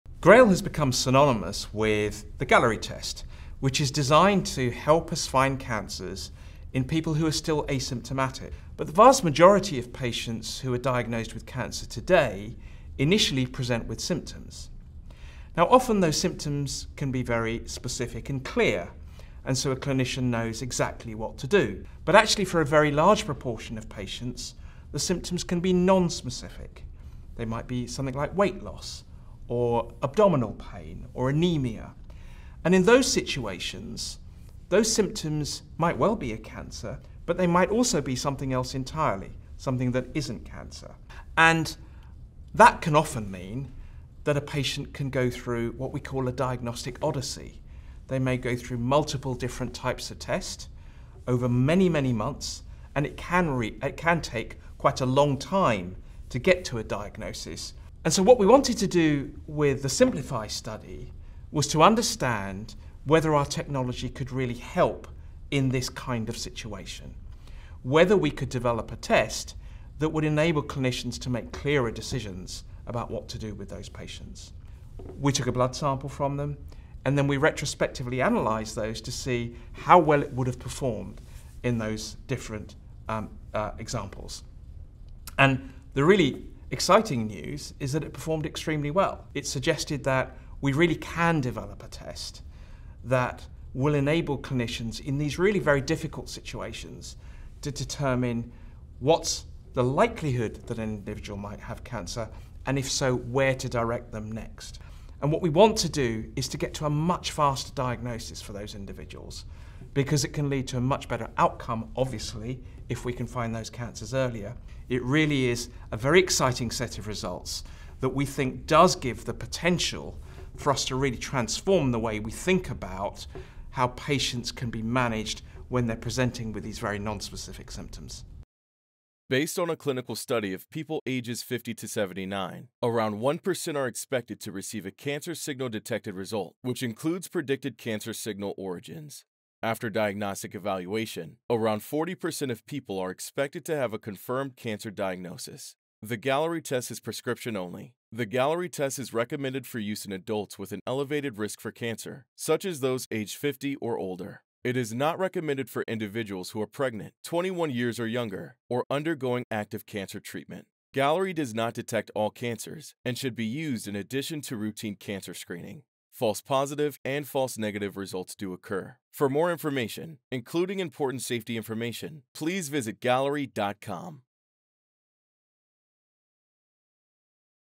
GRAIL's SYMPLIFY Study Results at ASCO 2023
At ASCO23, GRAIL and the University of Oxford present results from SYMPLIFY, which show strong performance of multi-cancer early detection testing in people presenting with non-specific symptoms.